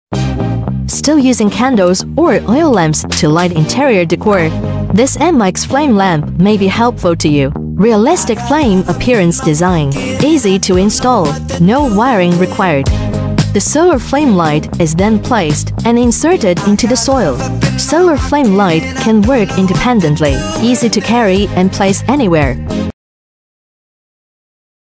Still need candles for ambience? Anmeix Solar Flame Light helps you!
Anmeix Solar Flame Lights

【LATEST FLAME DESIGN】Realistic flame appearance design with dynamic flickering lights, a perfect substitute for real flames, multiple combinations randomly placed, as if you are in a campfire atmosphere.
【PLUG-AND-PLAY】 Easy to install, no wiring required.Get the torch connected with the stake and spike, The solar flame light is then placed and inserted into the soil and ready for use. It also allows you to move them around whenever you want.
【SOLAR & LIGHT SENSOR SWITCHES】The solar flame lights are equipped with three solar panels distributed in three different directions so that it can receive sunlight at any angle. The light sensor automatically recognizes the switch, turns on automatically when it is dark and turns off by itself when it is light, no need to operate manually.
【WATERPROOF DESIGN】Our solar outdoor flame light is very durable, IPX65 waterproof and high temperature resistant, able to withstand all kinds of weather throughout the year.
【OCCASIONS WIDLY USE】 ANMEIX Solar flame light can work independently, easy to carry and place anywhere, suitable for lawn, garden, patio, yard, driveway, walkway, outdoor camping, beach party and so on. It can light up the environment as well as add to the atmosphere of the program and party.